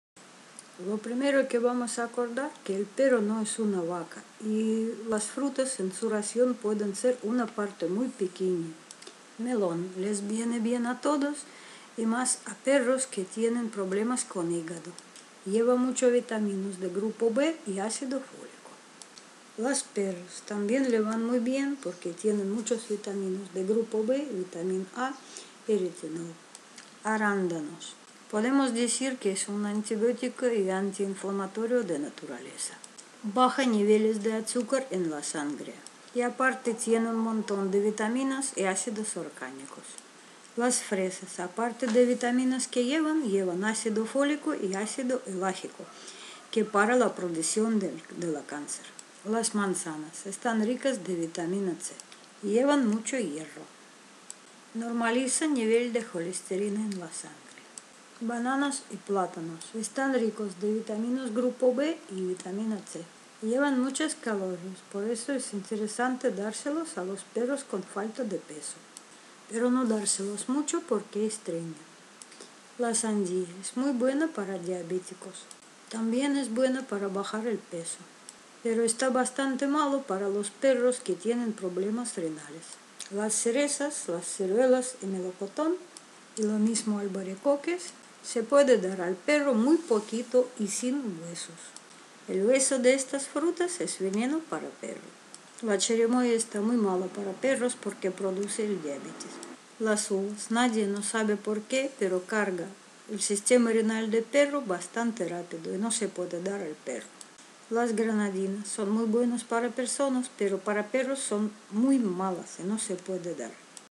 Frutas para el perro.
Algunas son venenosas para perro.¿Que fruta puede comer el perro?